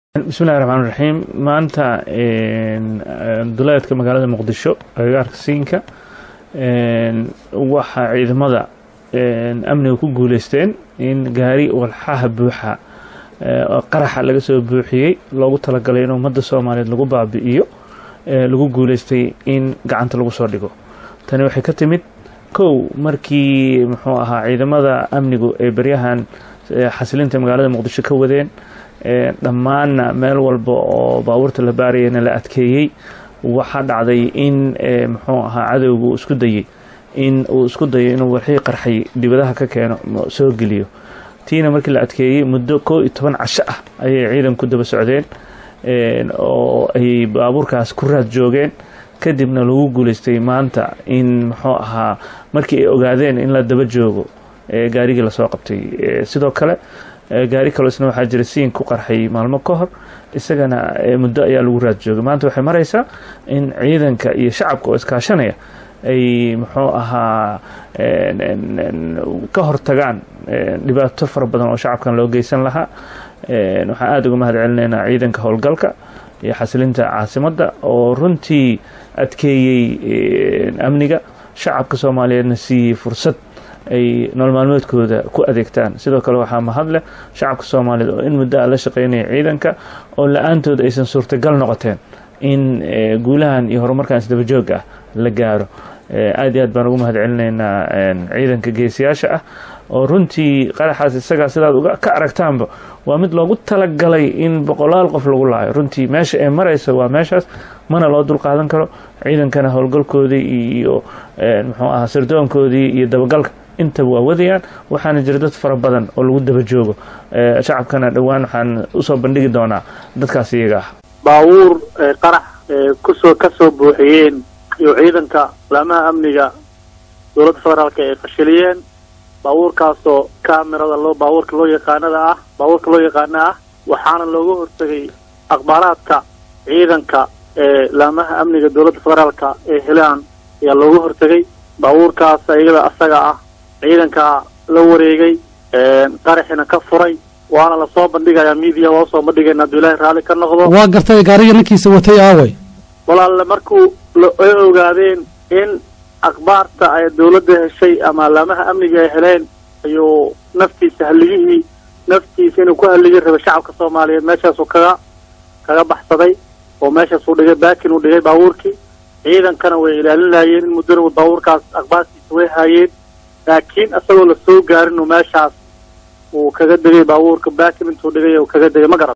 Gaari miineysan oo Muqdisho lagu qabtay
Ciidamada Nabada Somaliya ayaa gabtay gaari siday waxyaabaha qarxa ee alShabaab rabeen inay ku dhibaateyaan shacabka